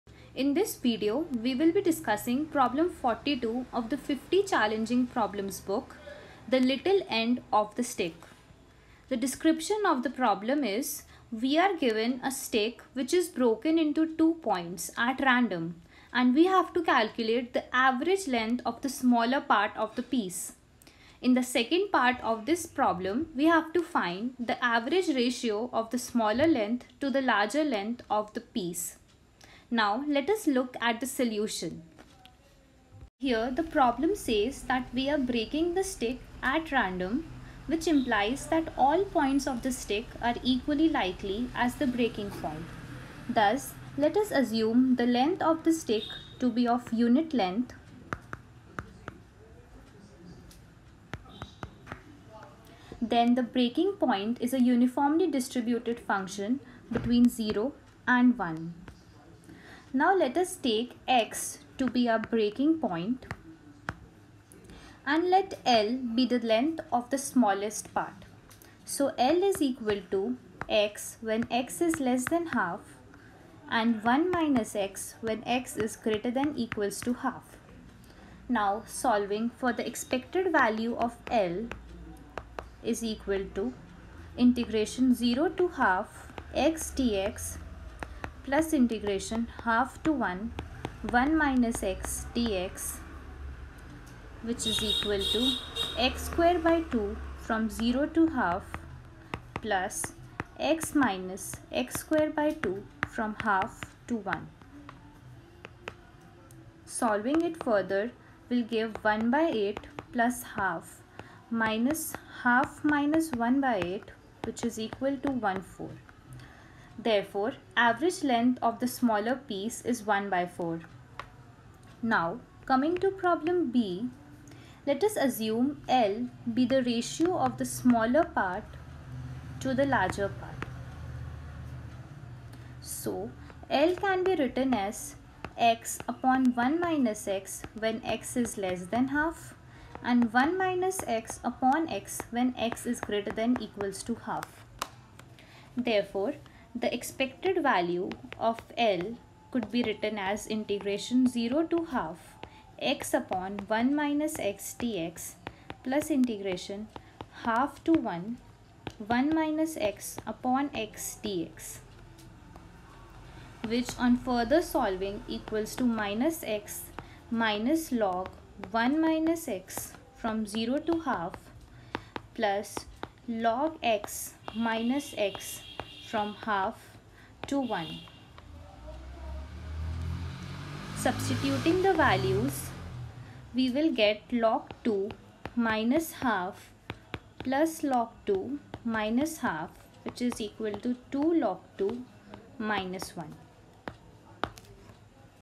42 : The Little End Of The Stick | Ace Quant | Probability Theory
Question 42 :
a) If a stick is broken in two at random, what is the average length of the smaller piece?
b) (For calculus students) What is the average ratio of the smaller length to the larger?